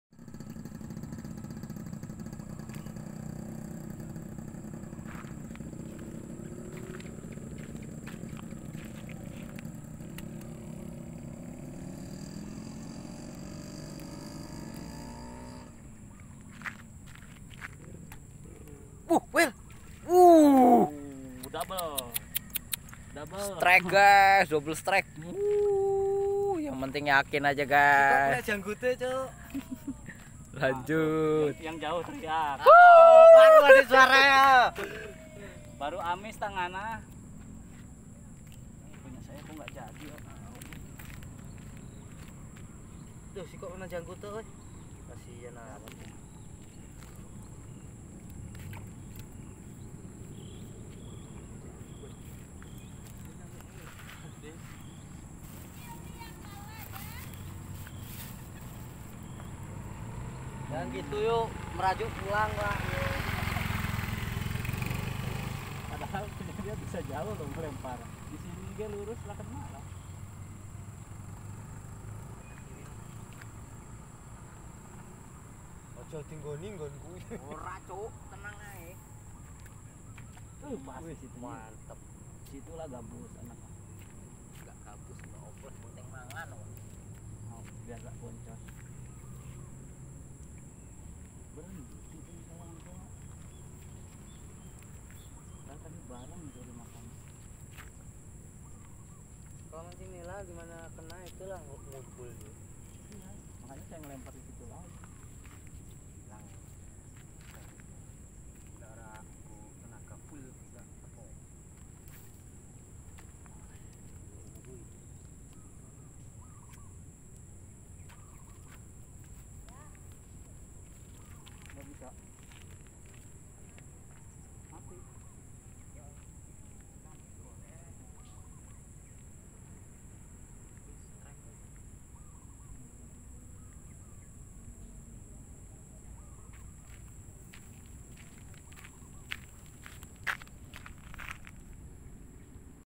mancing nila pas di sarangnya
Hallo Kawan kawan
kali ini kami mancing ikan nila pas di sarangnya
saksikan keseruan srike srike nya ya
semoga terhibur ya kawan kawan